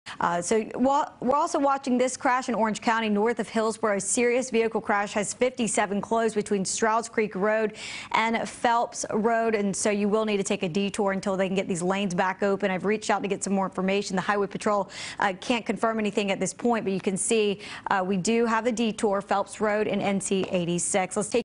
NC-57 closed near Hillsborough due to crash
The road is closed between Strouds Creek and Phelps roads.